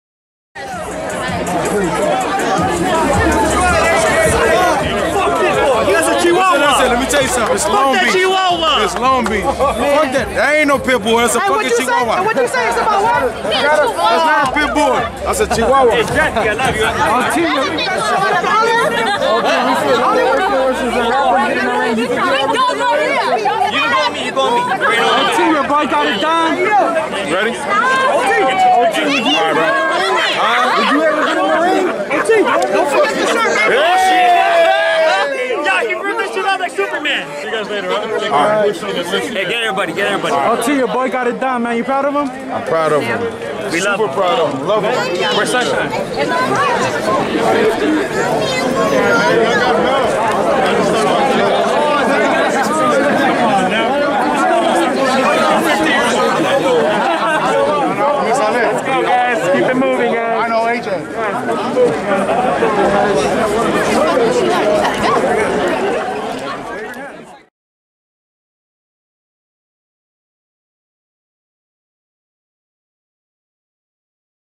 O.T. GENASIS REACTS TO AJ MCKEE CHOKING OUT PITBULL IN ROUND 1: "NOT A PITBULL...A CHIHUAHUA"
FightHype.com was on the scene at The Forum in Inglewood, California where AJ McKee became the new Bellator featherweight champion, dropping and choking out former champion Patricio Pitbull in the first round of the main event at Bellator 263. You don't want to miss what went down after the fight. Check it out!